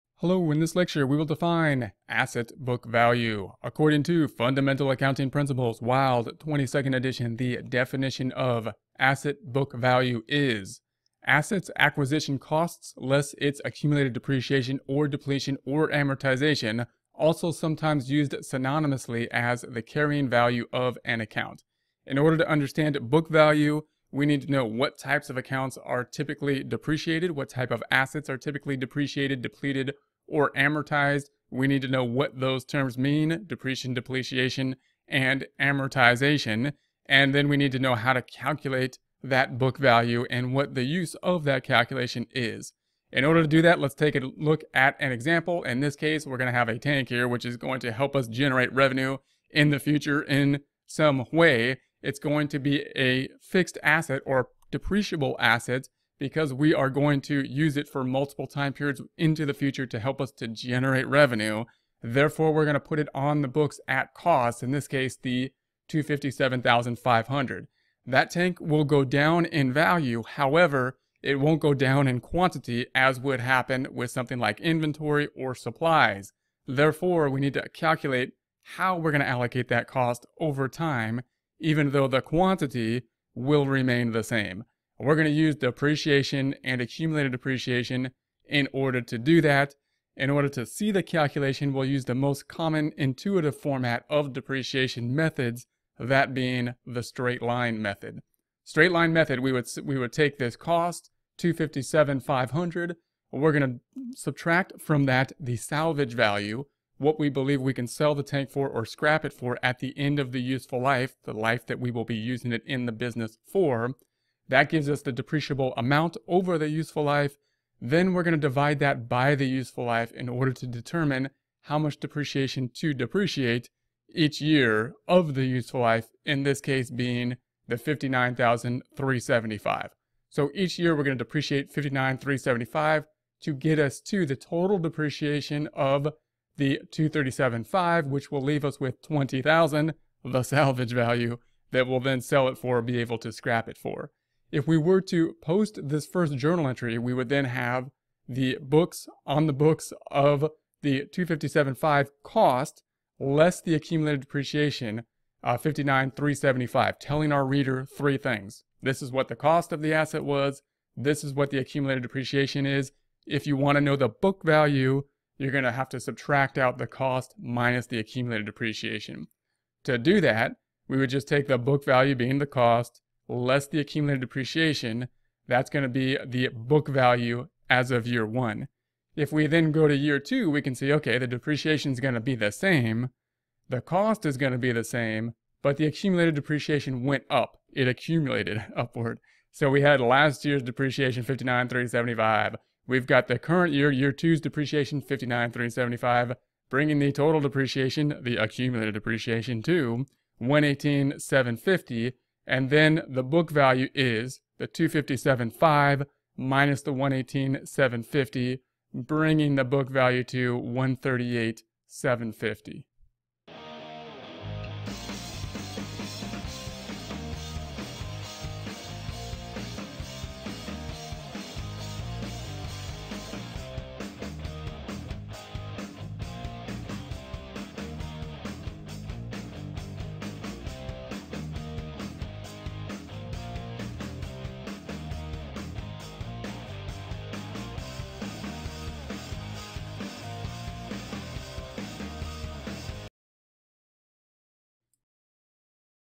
Asset Book Value Definition - What is Asset Book Value?
Asset book value definition including break down of areas in the definition. Analyzing the definition of key term often provides more insight about concepts. Asset book value can be defined as: Asset’s acquisition costs less its accumulated depreciation (or depletion, or amortization); also somethomes used synonymously as the carrying value of an account. Book value is commonly used when referring to fixed assets or depreciable assets, assets that have a relatively long useful life, these assets being put on the books at cost and then depreciated. The book value is the estimated value of the asset, the cost of the asset less the accumulated depreciation, accumulated depreciation being the allocation of cost over the life, the cost that has been expensed as depreciation expense.